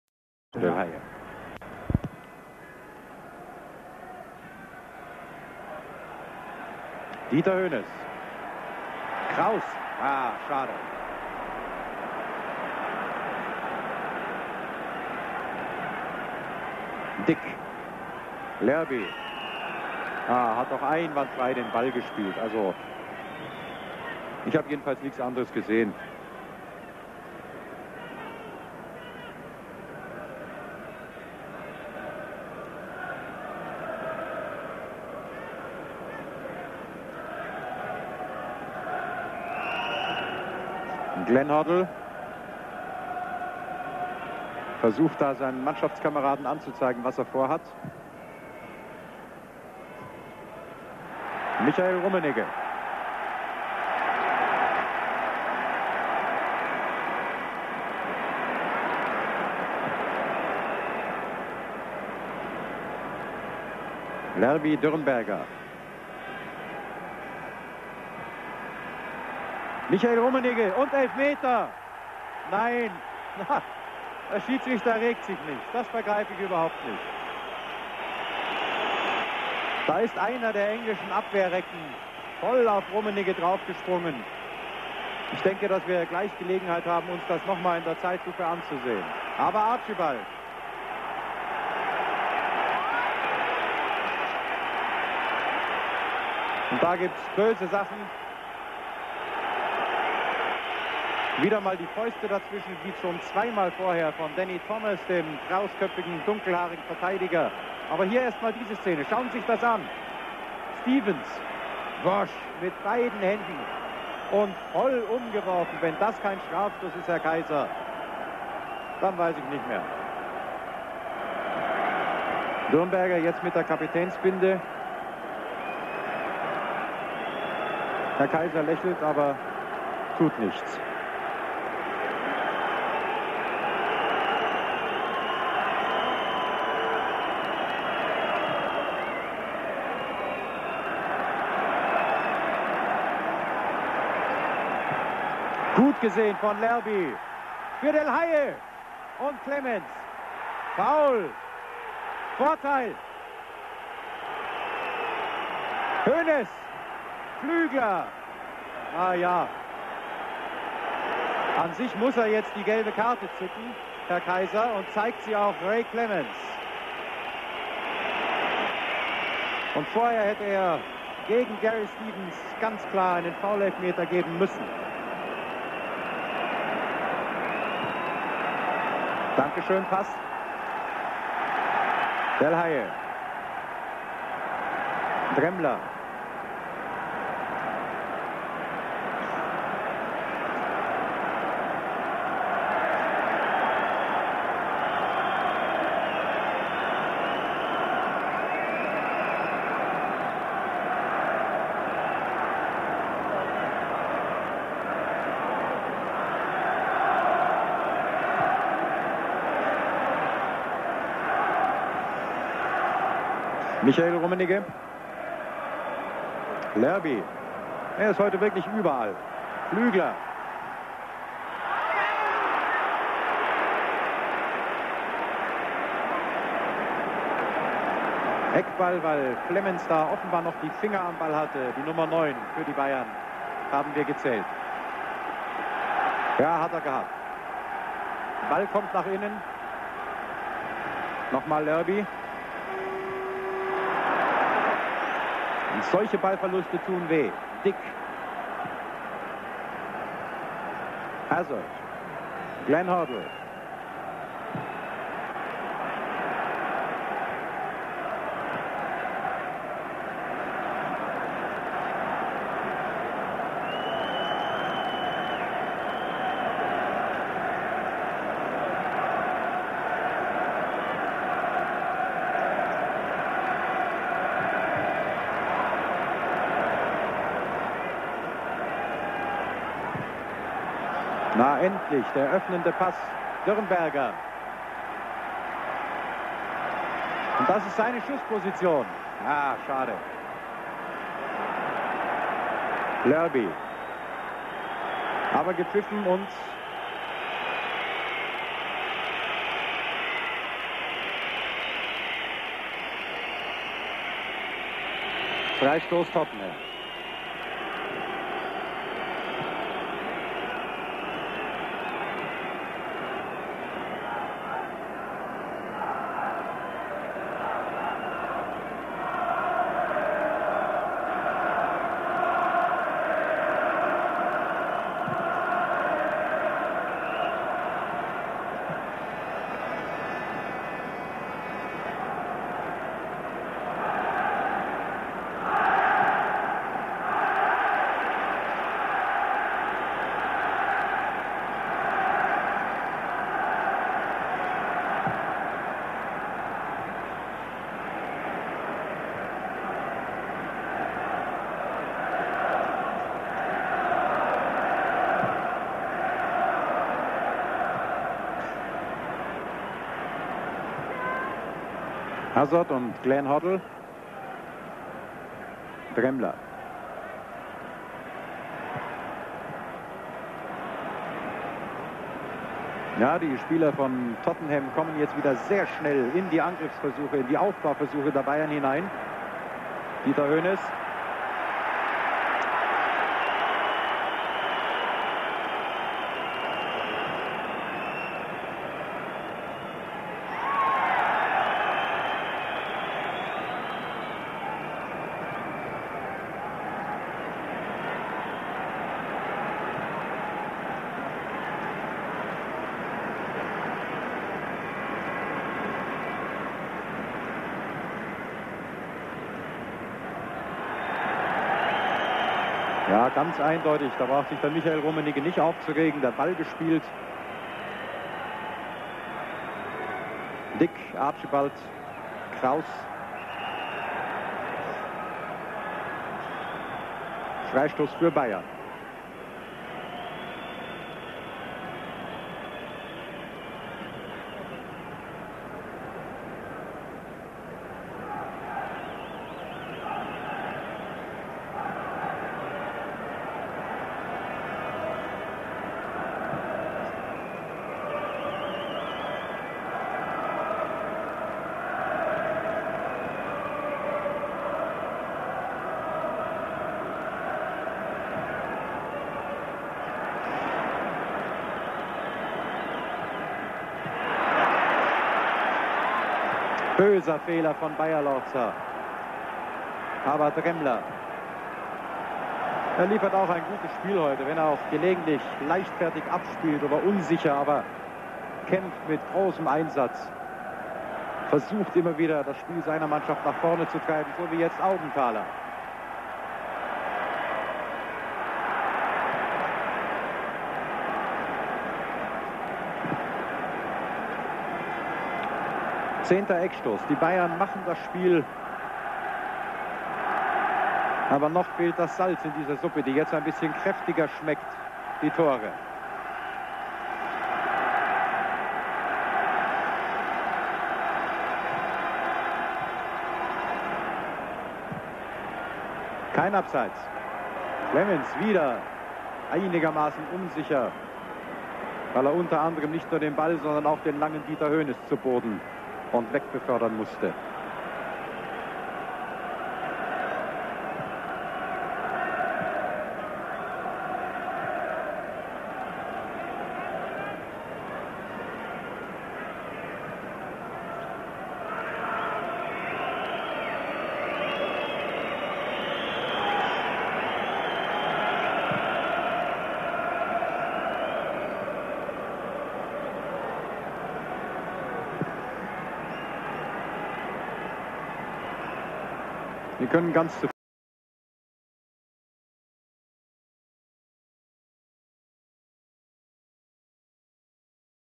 Bayern vs Tottenham (1983-84) (Part 1)
BAYERN MUNICH 1-0 TOTTENHAM HOTSPURS

Date: 23 November 1983
Competition: UEFA Cup, 3rd round, 1st leg
Referee: Keizer (Holland)
Attendance: 20,000

Bayern:
Pfaff - Dremmler, Augenthaler, Beierlorzer, Dürn-
berger - Kraus, Lerby, Pflügler - M.Rummenigge, D.Hoeness, K.H.Rummenigge (46. Del'Haye)

Tottenham:
Clemence - Hughton, G.Roberts, G.A.Stevens, Thomas - Perryman, Hazard (77. Brazil), Hoddle, Dick (77. Brooke) - Archibald, Falco